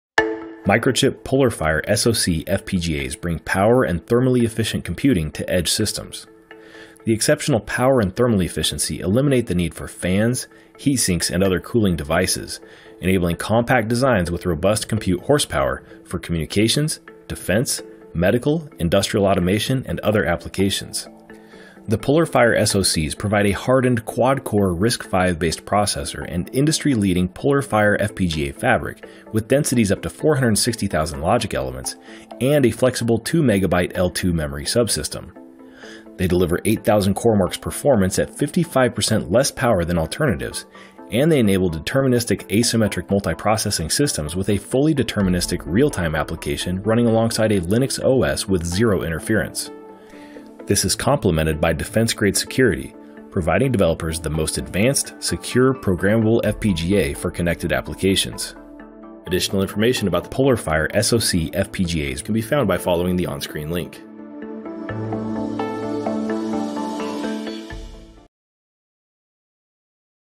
Microchip Technology: PolarFire® SoC FPGAs
PolarFire® SoC FPGAs cut static power consumption by 50% and deliver both thermal efficiency and defence-grade security for smart, connected systems. Our innovative SoC FPGA features a deterministic, coherent RISC-V CPU cluster and a deterministic L2 memory subsystem, enabling the creation of Linux® and real-time applications. Whether you're developing solutions for industrial, aerospace, communications, or any other application that demands a combination of power efficiency, performance and security, our PolarFire® SoC FPGA is the answer.